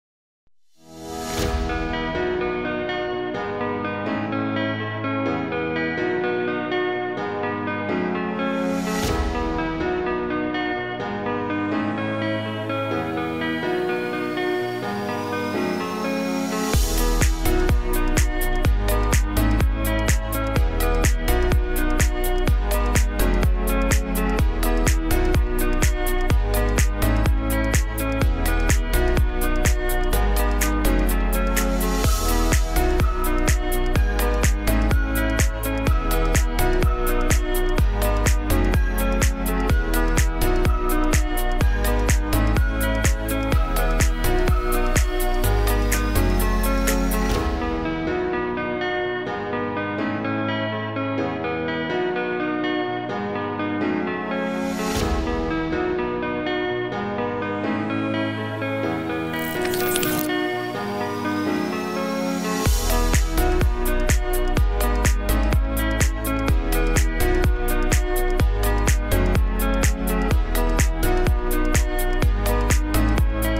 محرز محبوب جماهير السيتي
الدولي الجزائري رياض محرز محبوب جماهير السيتي الإنجليزي...وعدم مشاركته في نهائي أبطال أوروبا لم تؤثر على شعبيته والدليل هذا الفيديو..